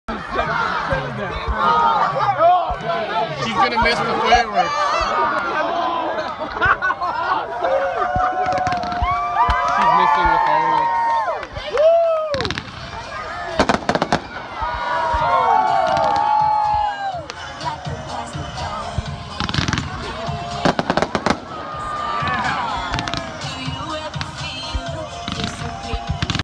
Carin Keland taking pictures and fireworks go boom!
Wisconsin. Fireworks.